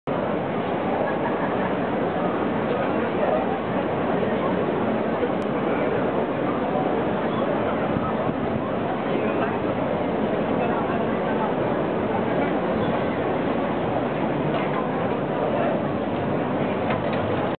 Soutien à Bordeaux 2013
Courte vidéo tournée le 1er septembre place Pey Berlan à Bordeaux à 19h00 à l'occasion de la visite du Jury